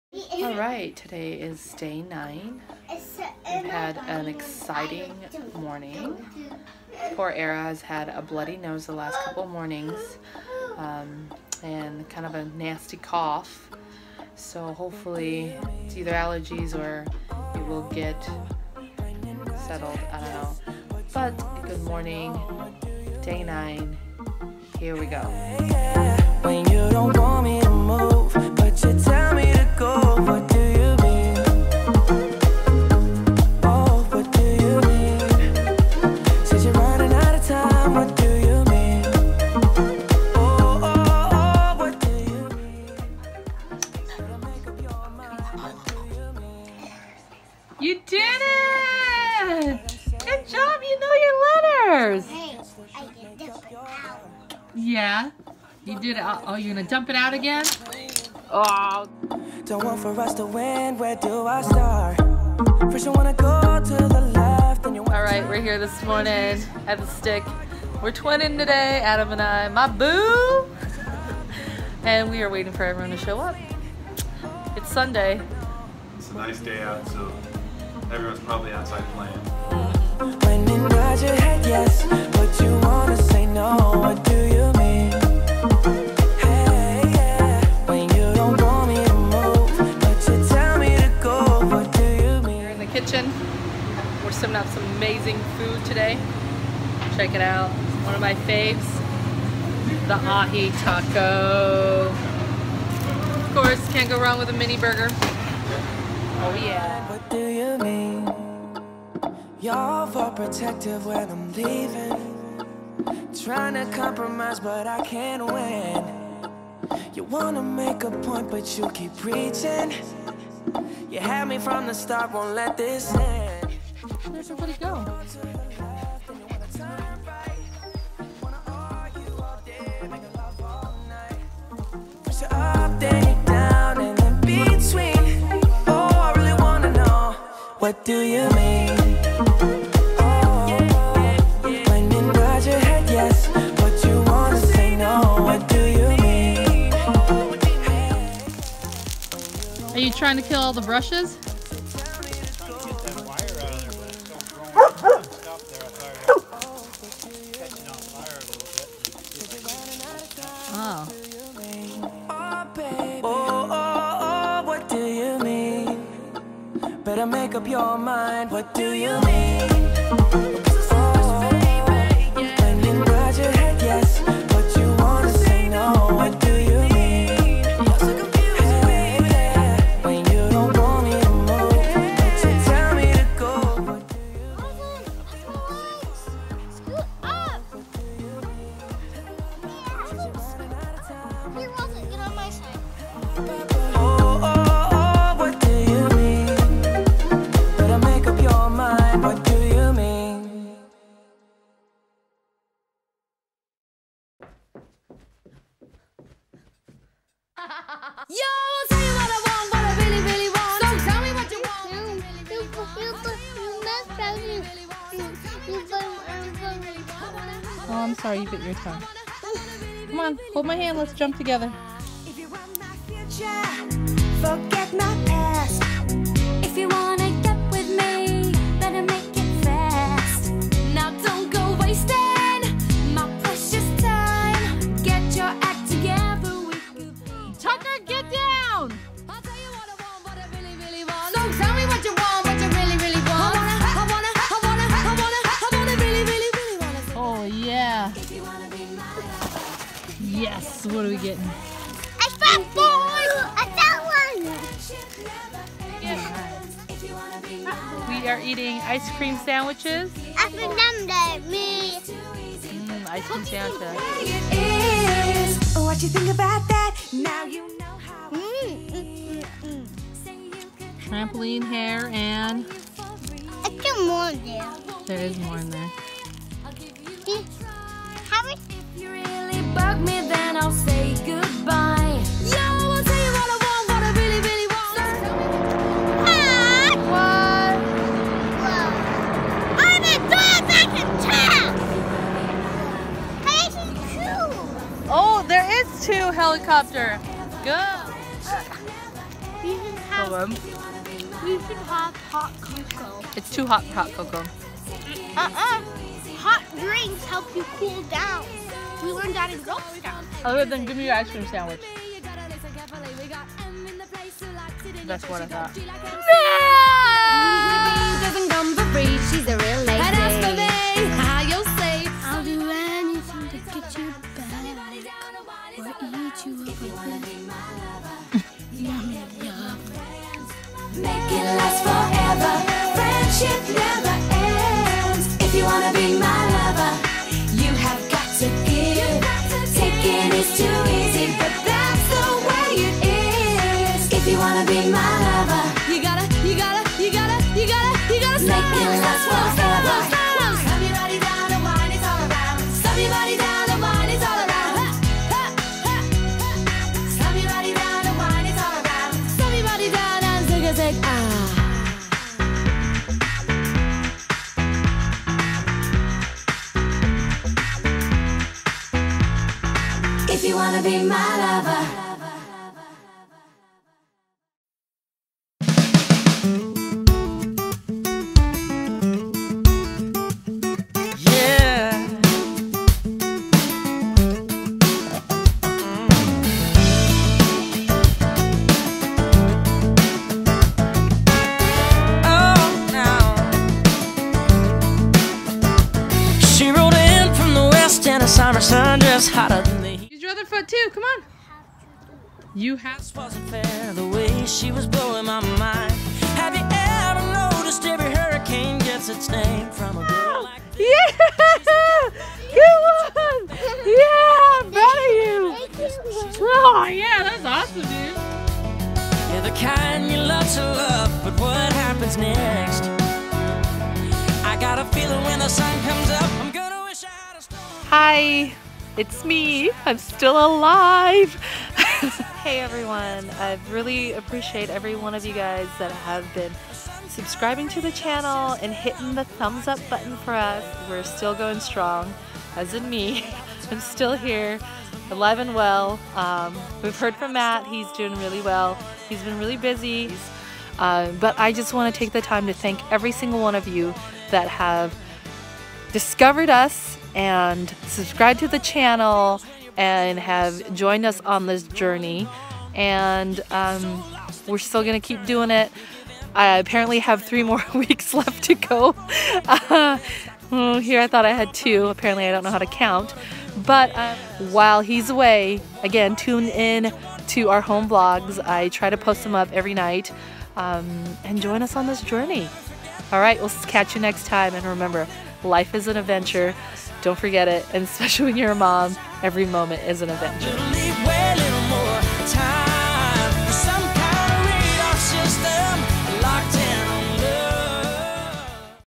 Home Vlog- Day 9
Sunshine, trampoline, ice cream sandwiches, and good company makes a perfect relaxing Sunday!